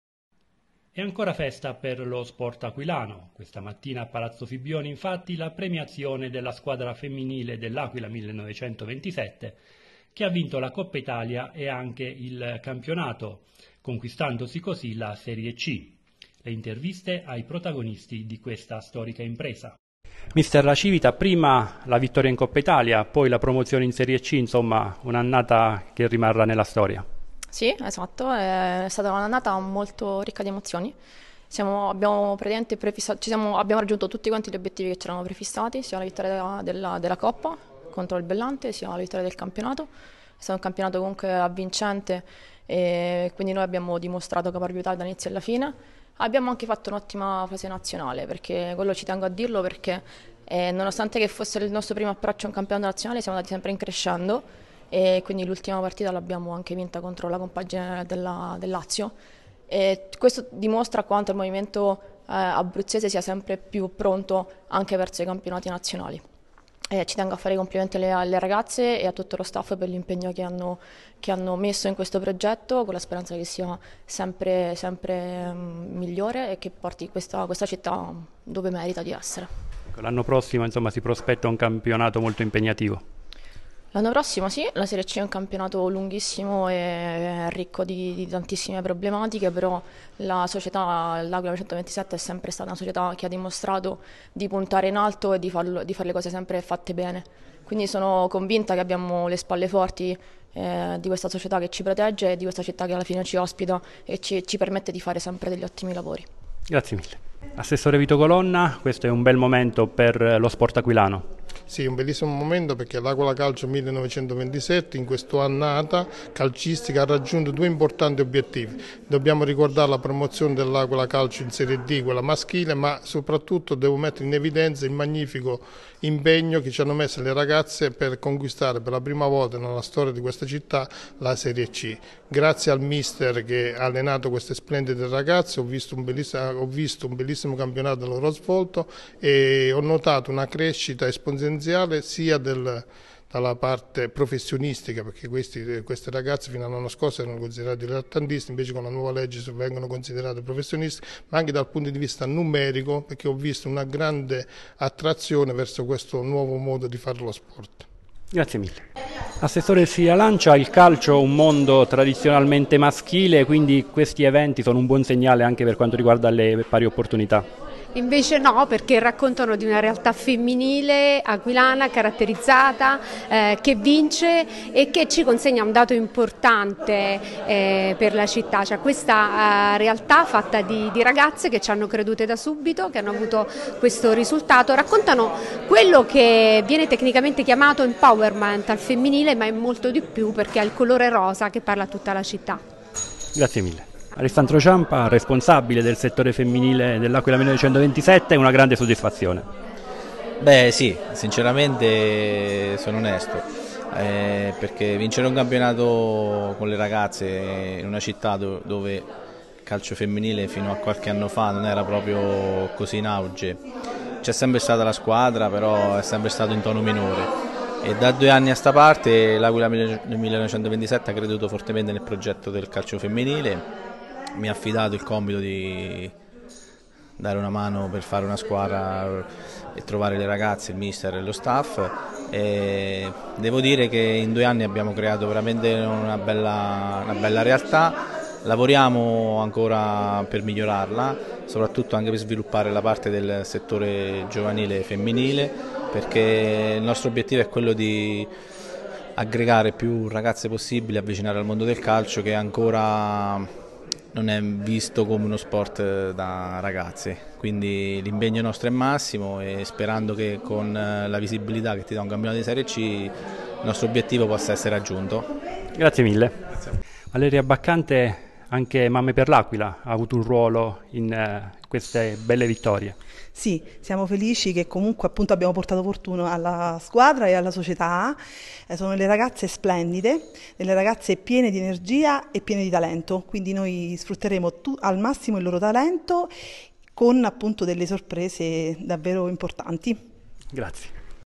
La squadra femminile dell'Aquila 1927 premiata per la promozione in serie C
A Palazzo Fibbioni la cerimonia per la premiazione della squadra femminile dell'Aquila 1927 dopo la vittoria in Coppa Italia e la promozione in serie B. Le interviste